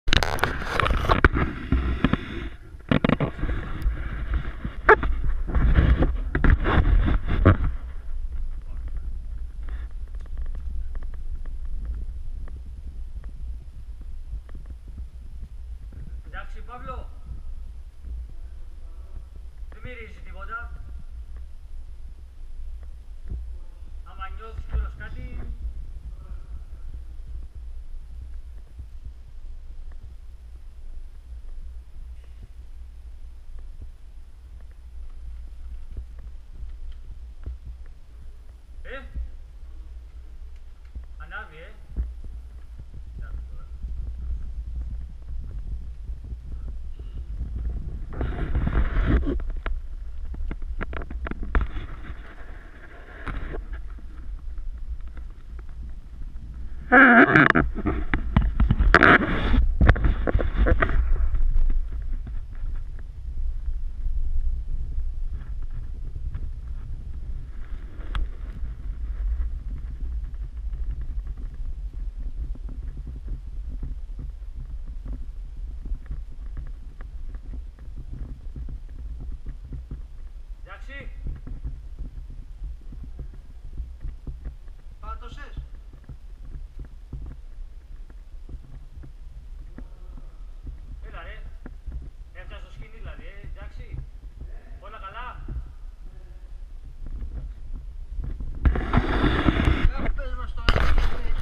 Μέσα στο μεταλλευτικό πηγάδι στην Παλαιά Φώκαια - Ανάβυσσο
Ο φίλος Παύλος Καδάς έχει κατέβει το πηγάδι